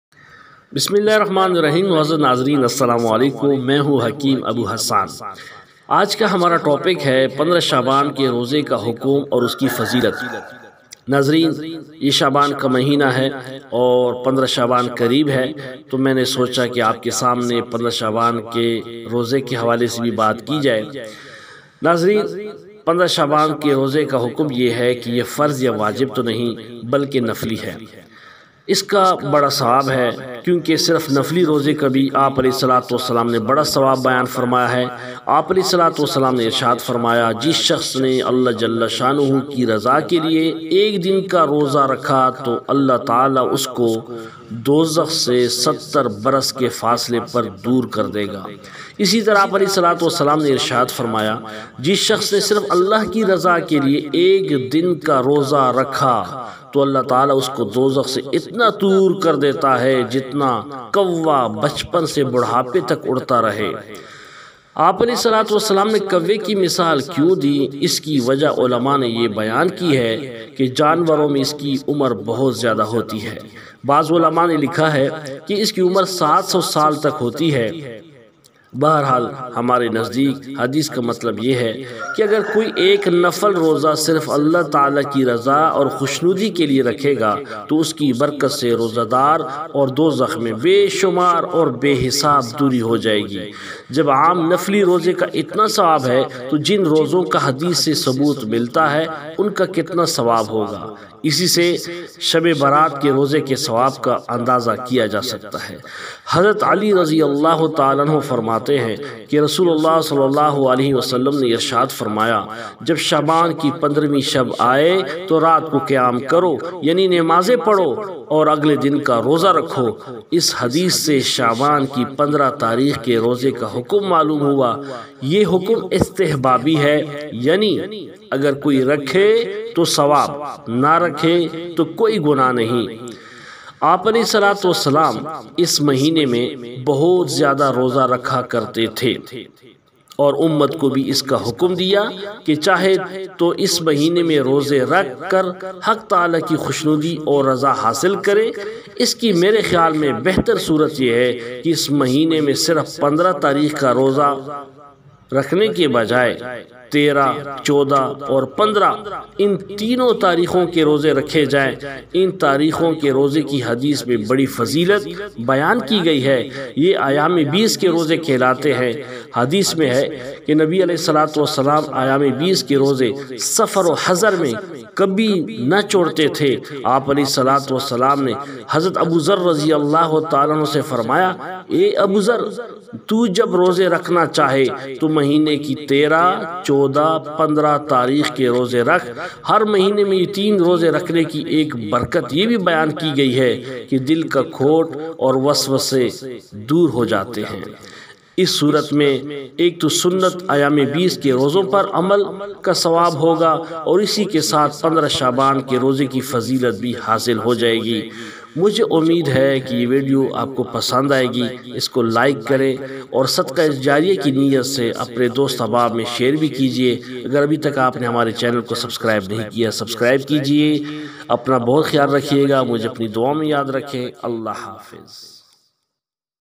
شبِ برات کی فضیلت واہمیت اور عبادت!!15شعبان کا اہتمام کیسے کیا جائے!!حکیم ابو حسان
شب برات#شعبان